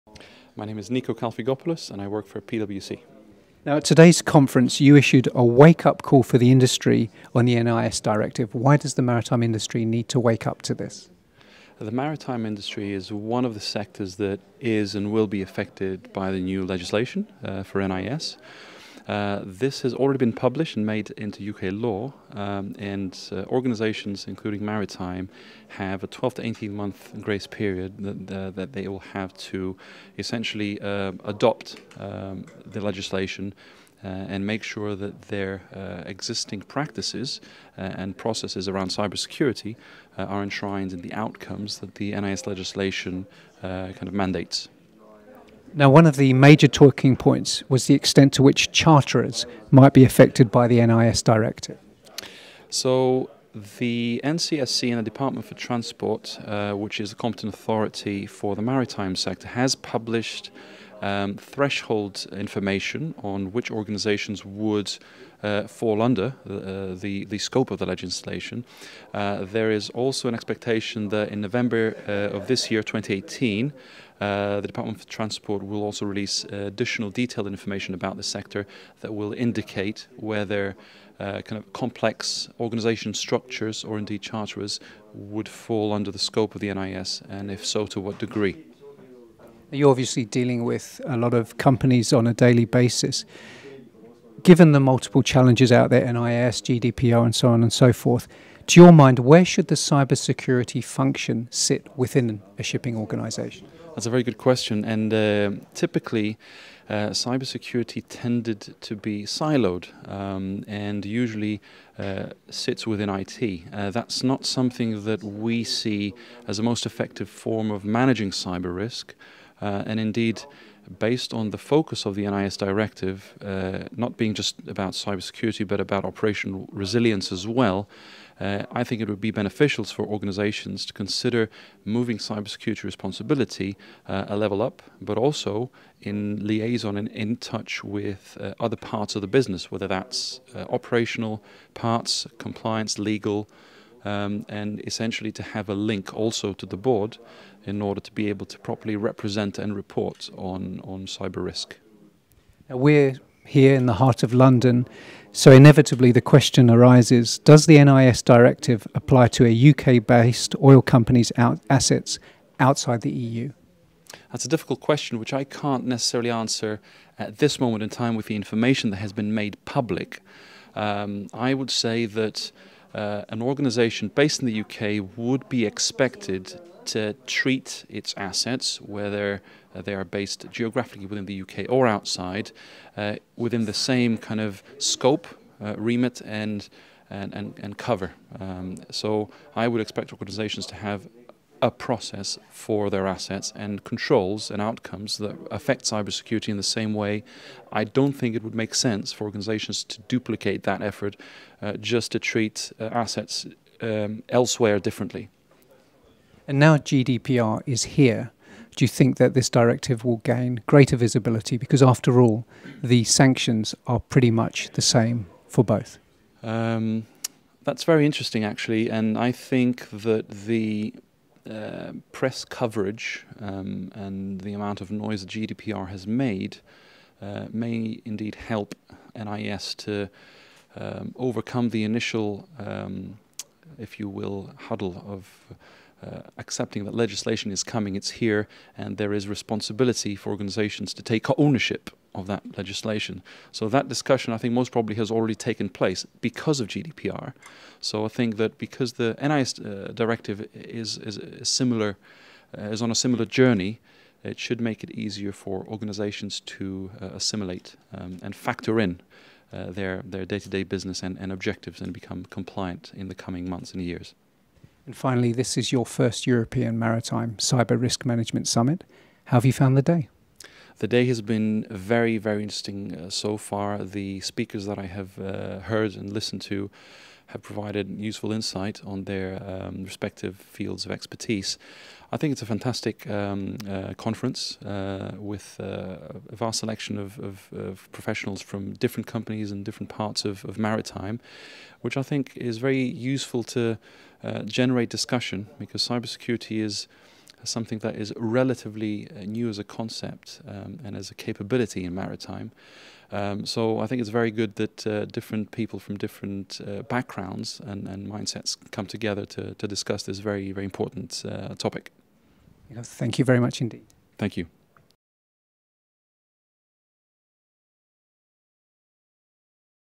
NIS directive: maritime industry has 12- 18 months to adopt legislation
PWC cyber security director Niko Kalfigkopoulos explains how the NIS directive is not just about cyber security, but also operational resilience